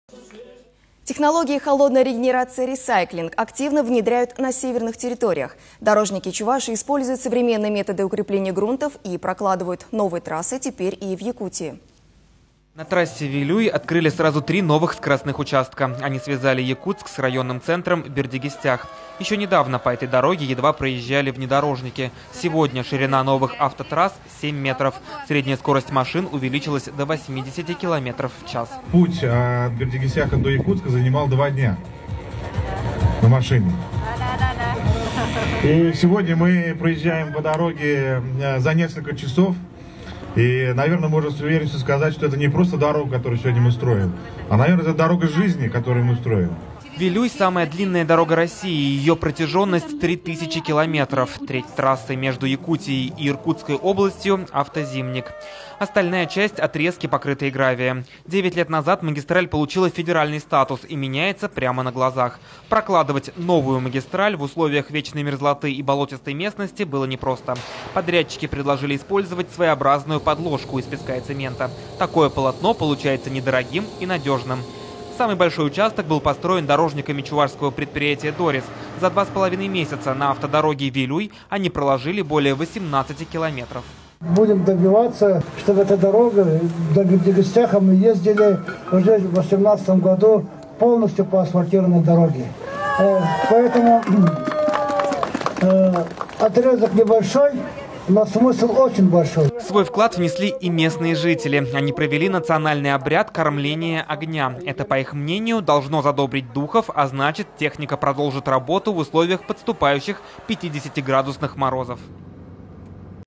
Чувашские дорожники используют современные методы укрепления грунтов и прокладывают трассы в Якутии
Технологии холодной регенерации «ресайклинг» активно внедряют на северных территориях.